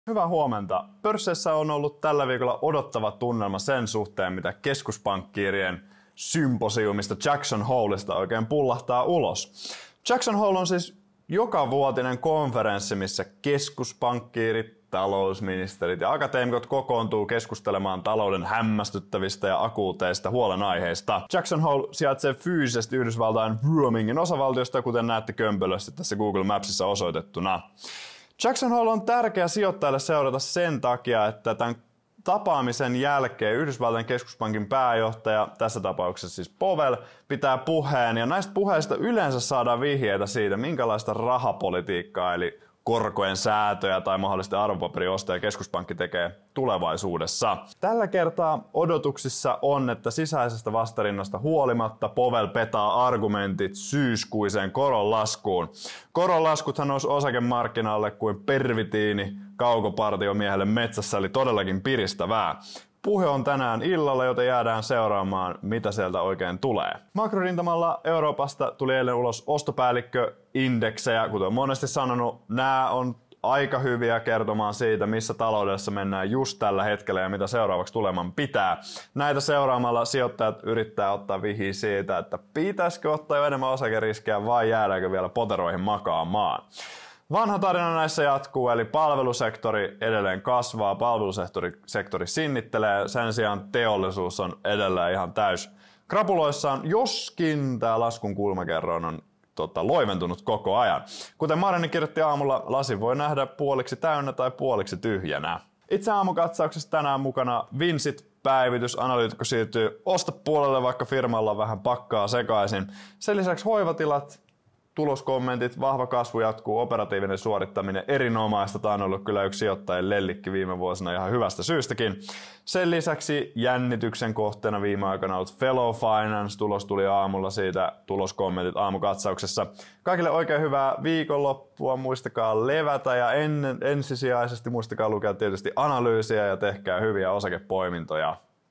Jackson Hole markkinan fokuksessa
Mikä on Jackson Hole ja miksi se on tärkeää? – Powellilta odotetaan petaamista koronlaskua varten syyskuussa – Euroopan ostopäällikköindeksit helpotus, vaikka teollisuus onkin vaikeuksissa – Aamarissa mm. Vincit, Hoivatilat ja Fellow Finance